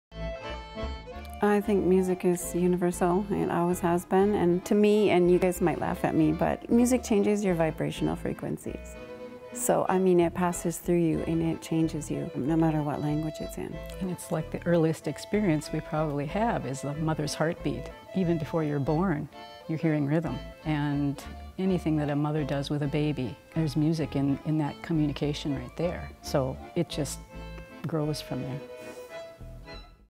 Music Profiles - Caleigh - 914
The band Caleigh talk about the impact music has on our lives.